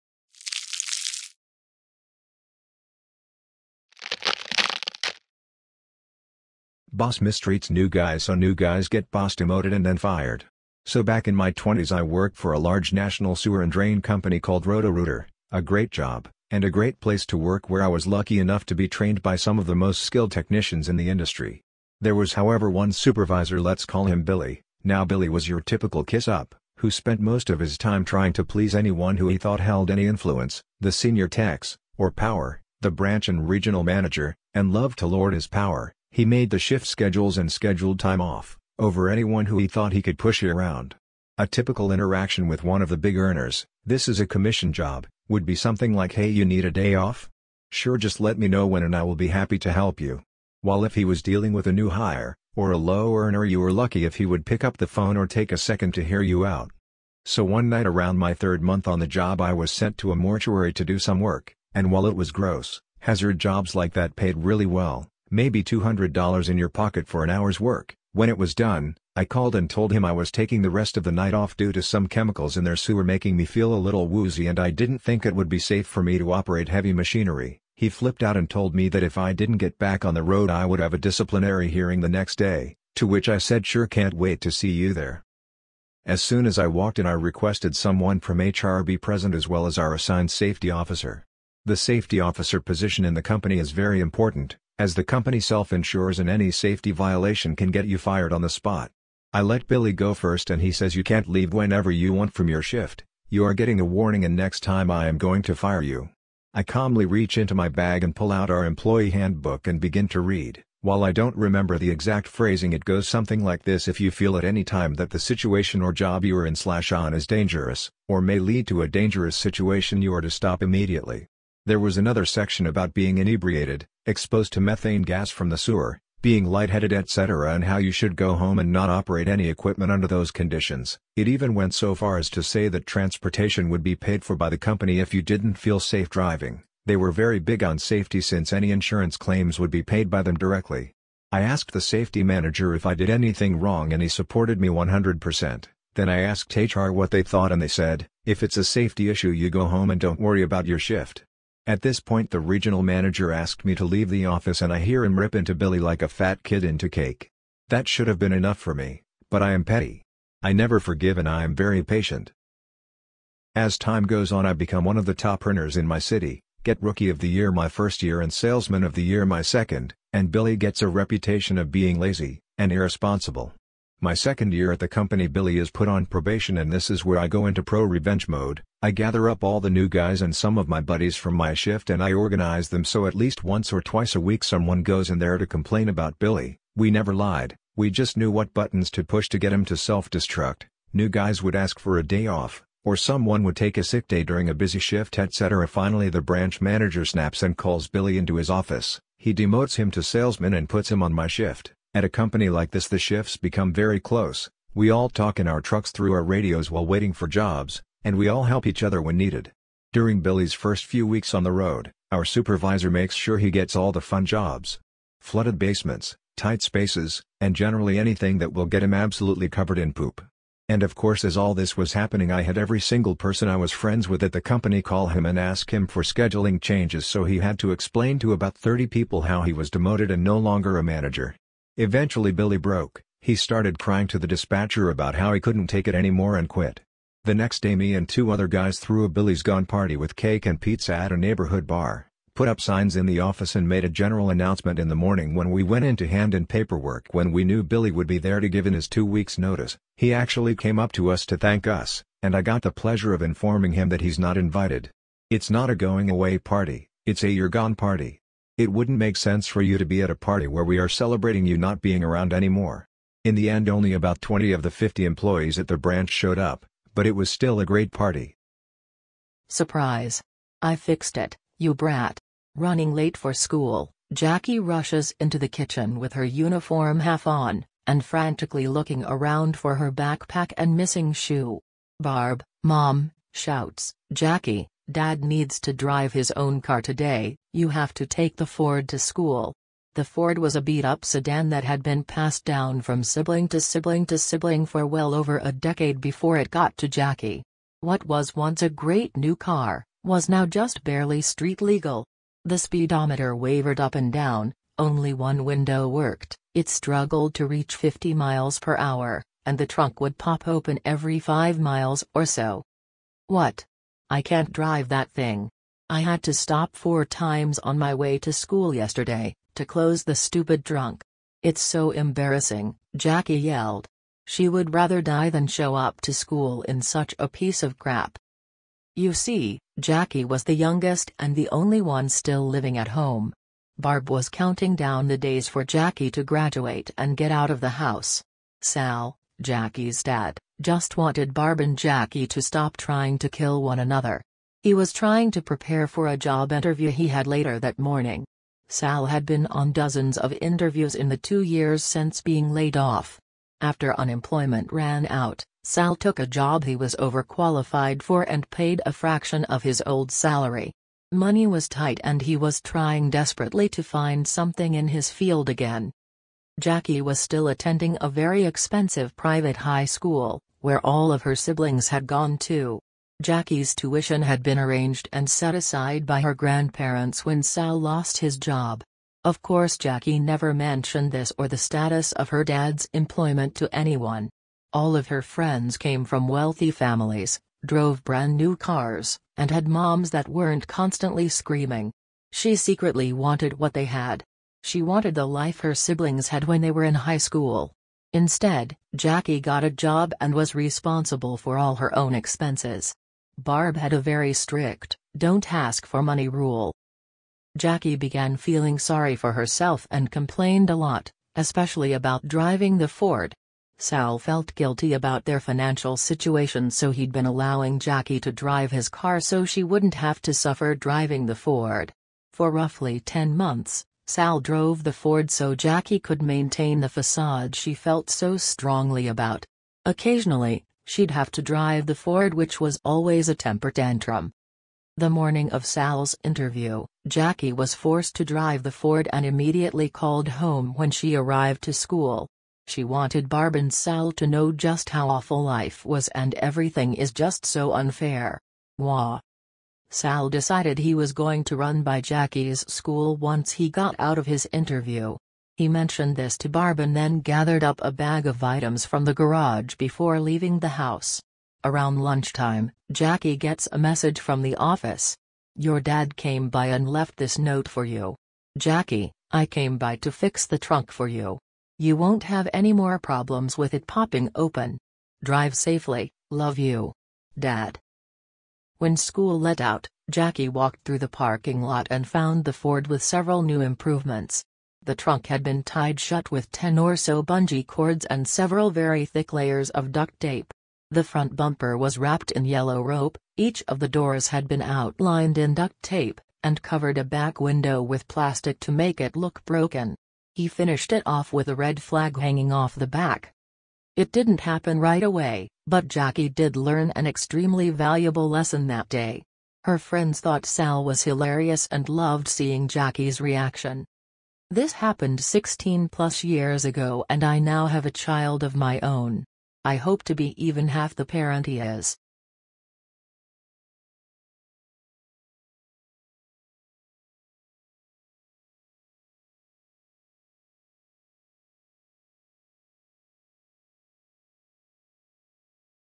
Reddit Pro Revenge on Boss r/ProRevenge #1
Boss mistreats new guys so new guys get boss demoted and then fired.

r/proRevenge Reddit